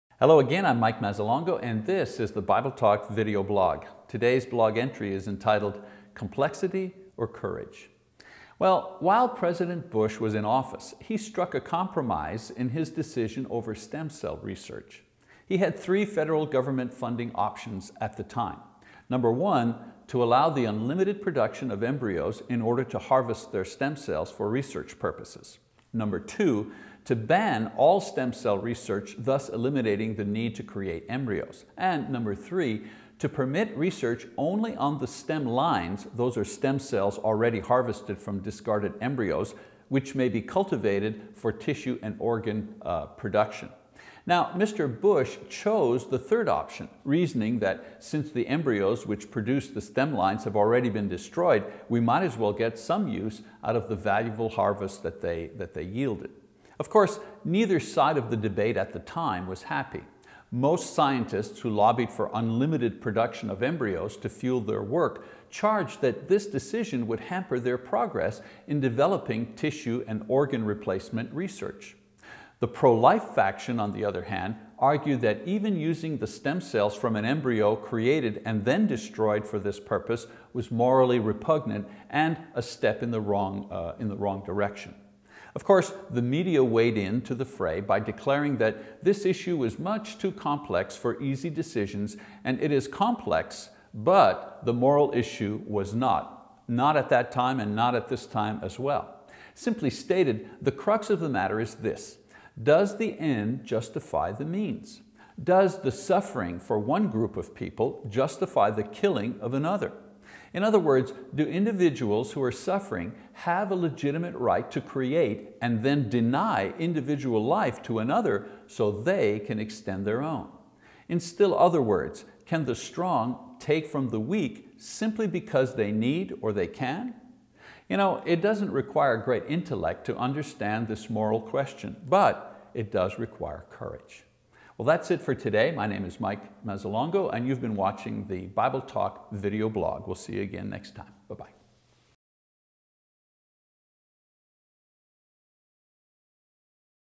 Complexity or Courage? – Mike Mazzalongo | BibleTalk.tv
President Bush struck a compromise in his decision over stem cell research. He had three Federal government funding options.